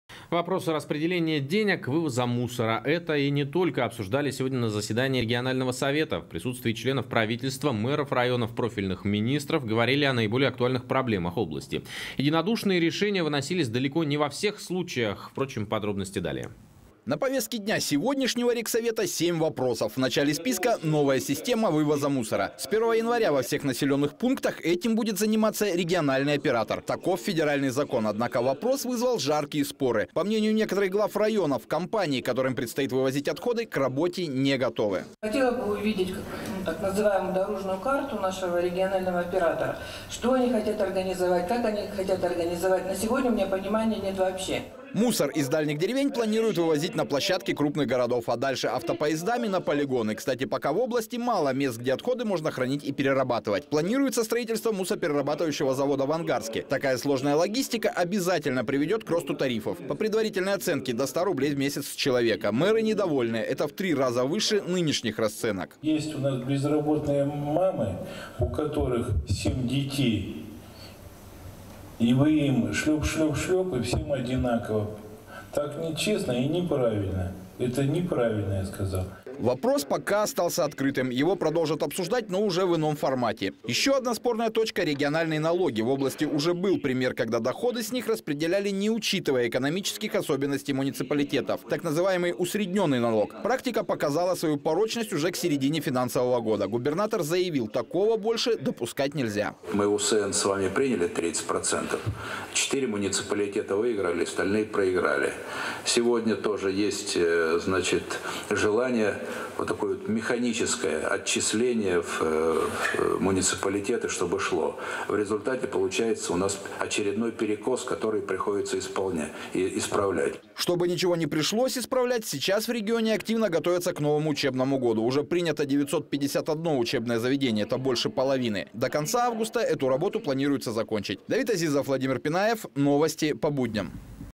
Мусороперерабатывающий завод планируется построить в Ангарске
Вопросы распределения денег, вывоза мусора... Это и не только обсуждали сегодня на заседании регионального совета. В присутствии членов правительства, мэров районов, профильных министров говорили о наиболее актуальных проблемах области. Единодушные решения выносились далеко не во всех случаях. Подробности - далее.

На повестке дня сегодняшнего регсовета - семь вопросов. В начале списка - новая система вывоза мусора. С 1 января во всех населённых пунктах этим будут заниматься региональные операторы. Таков федеральный закон. Однако вопрос вызвал жаркие споры. По мнению некоторых глав районов, компании, которым предстоит вывозить отходы, к работе не готовы.

ТАМАРА КЛИМИНА, мэр Усть-Кутского района: "Хотелось бы видеть так называемую "дорожную карту" нашего регионального оператора. Что они хотят организовать? Как это они хотят организовать? На сегодня у меня понимания нет вообще".

Мусор из дальних деревень планируют вывозить на площадки крупных городов, а дальше автопоездами на полигоны. Кстати, пока в области мало мест, где отходы можно хранить и перерабатывать. Планируется строительство мусороперерабатывающего завода в Ангарске. Такая сложная логистика обязательно приведёт к росту тарифов, по предварительной оценке, до 100 рублей в месяц с человека. Мэры не довольны, это в три раза выше нынешних расценок.

АНДРЕЙ ПОЛОНИН, мэр Куйтунского района: "Есть у нас безработная мама, у которой 7 детей. А вы им - шлёп-шлёп, и всем одинаково. Так нечестно и неправильно. Это неправильно, я сказал".

Вопрос пока остался открыт, его продолжат обсуждать, но уже в уже в ином формате. Ещё одна спорная точка - региональные налоги. В области уже был пример, когда доходы с них распределяли, не учитывая экономических особенностей муниципалитетов, так называемый "усредненный налог". Практика показала свою порочность уже к середине финансового года. Губернатор заявил такого больше допускать нельзя.

СЕРГЕЙ ЛЕВЧЕНКО, губернатор Иркутской области: "Мы УСН приняли с вами 30 процентов. 4 муниципалитета выиграли, остальные проиграли. Сегодня тоже есть желание, механическое отчисление в муниципалитеты чтобы шло. В результате получается очередной перекос, который приходится исправлять".

Чтобы ничего не пришлось исправлять - сейчас в регионе активно готовятся к новому учебному году. Уже принято 951 учебное заведение - это больше половины. До конца августа эту работу планируется закончить.